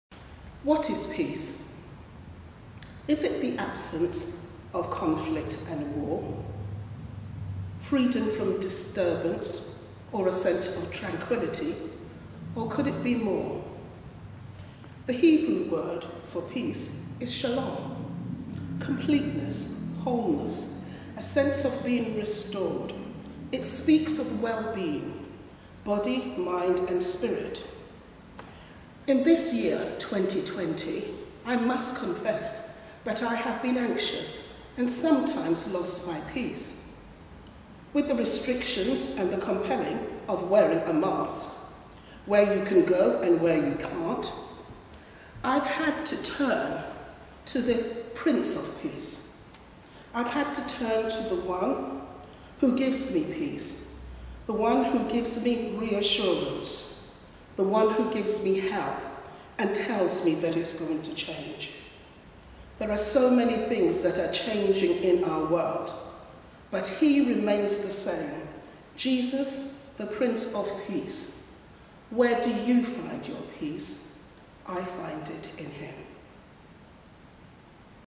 What is Peace? Christmas Message
A short Christmas message from our Church Worker Rev Janet Graham entitled: What is Peace?
Created as part of the Churches Together in Hitchin video for Screen In The Square over the 19th & 20th December weekend